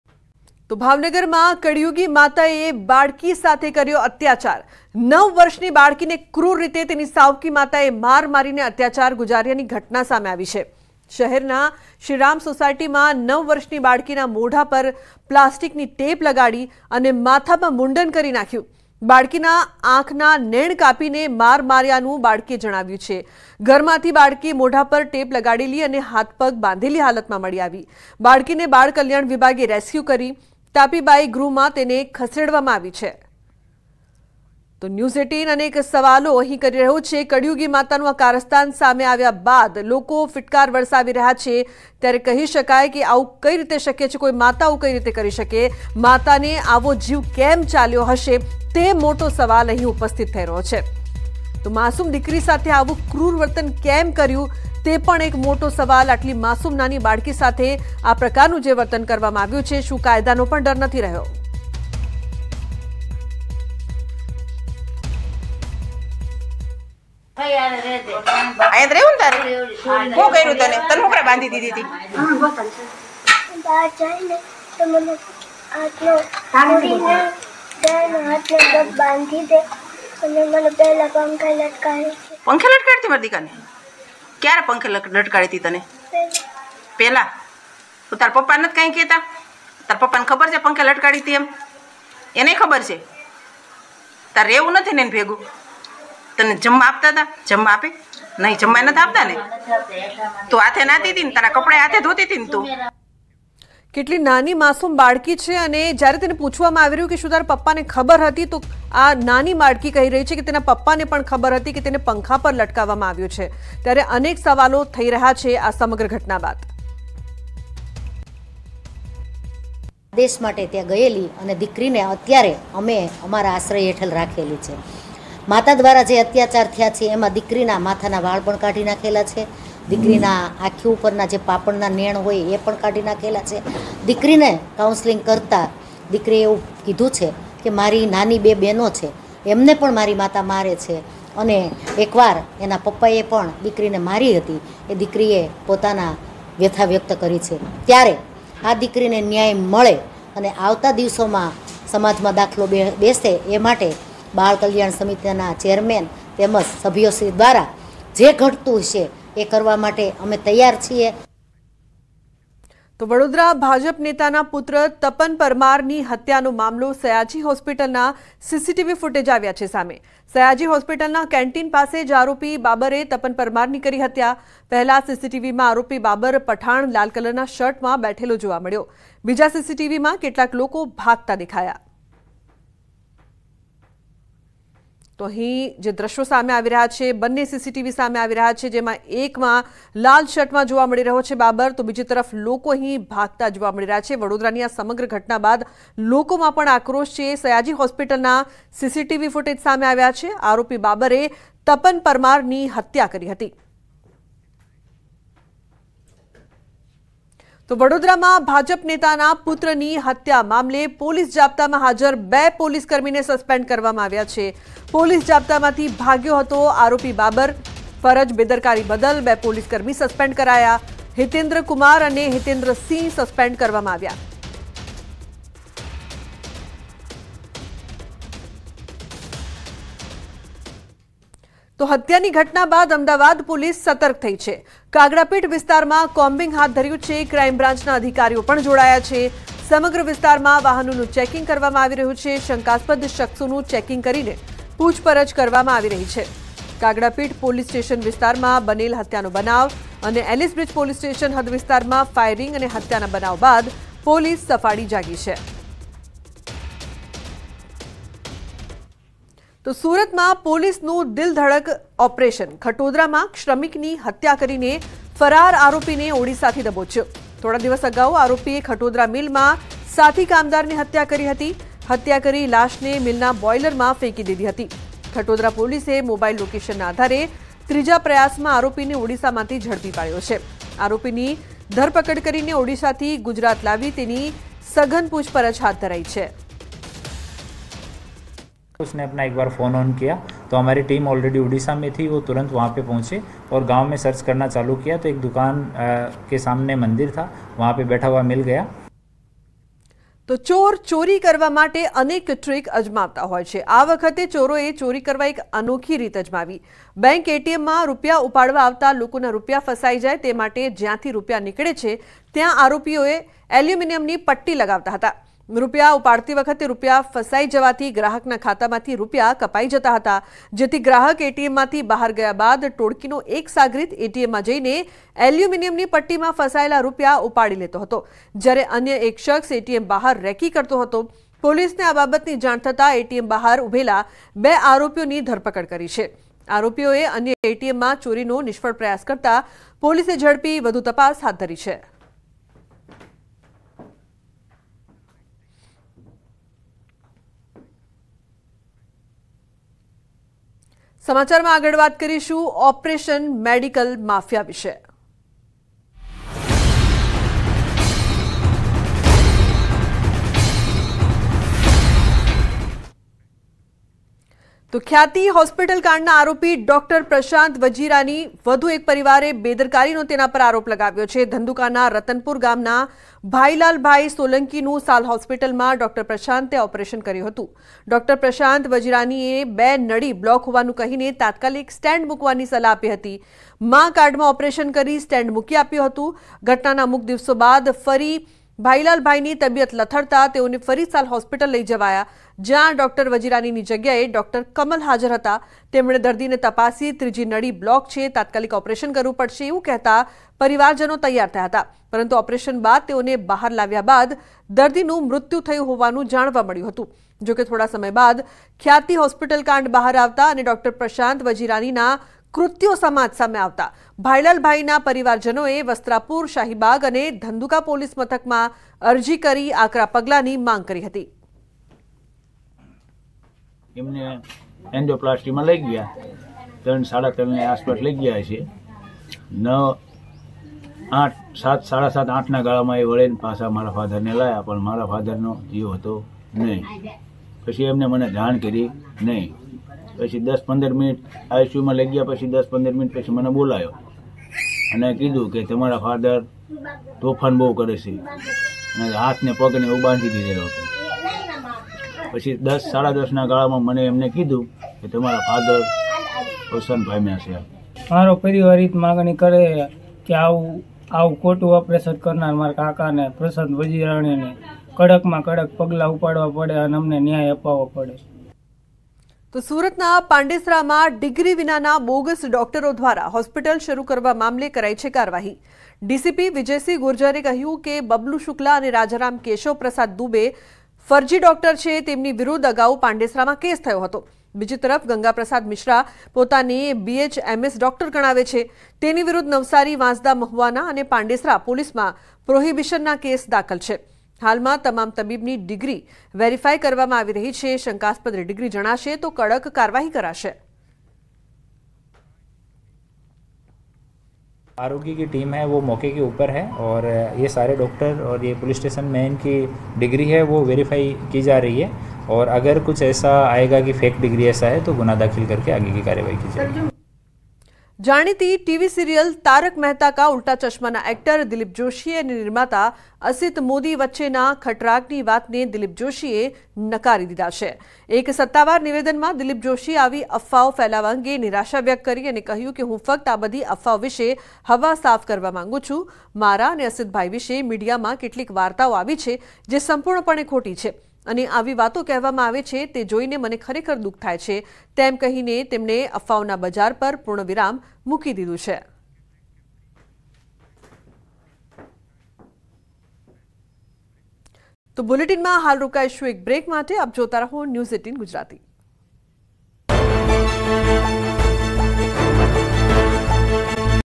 Bhavnagar News : ભાવનગરમાં સાવકી માતાએ બાળકી સાથે કર્યો અત્યાચાર | Gujarati Samachar | News18
Bhavnagar News: Stepmother tortured a girl in Bhavnagar. A nine-year-old girl was brutally beaten and tortured by her stepmother. A nine-year-old girl was brutally beaten and tortured by her stepmother.

Bhavnagar News : ભાવનગરમાં સાવકી માતાએ બાળકી સાથે કર્યો અત્યાચાર. નવ વર્ષની બાળકીને ક્રૂર રીતે તેની સાવકી માતાએ માર મારી અત્યાચાર ગુજાર્યાની ઘટના સામે આવી. નવ વર્ષની બાળકીને ક્રૂર રીતે તેની સાવકી માતાએ માર મારી અત્યાચાર ગુજાર્યાની ઘટના સામે આવી. n18oc_samachar